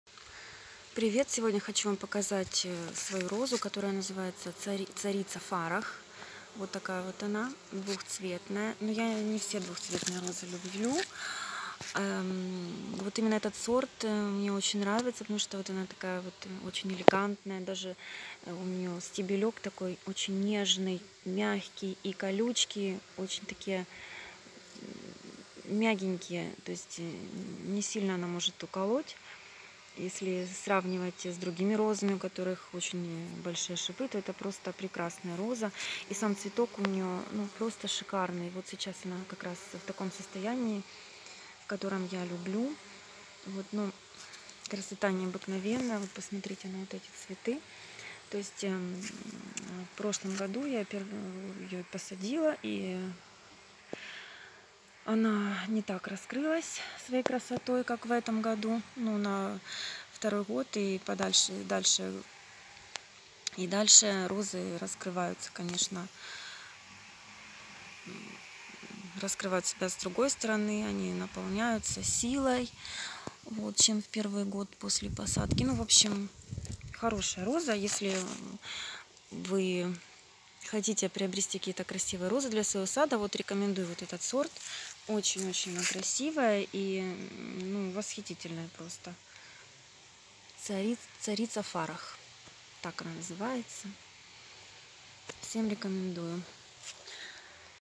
Шикарная роза сорта " Императрица Фарах "Imperatrice Farax rose
Хочу показать вам свою розу  сорта " Императрица  Фарах ".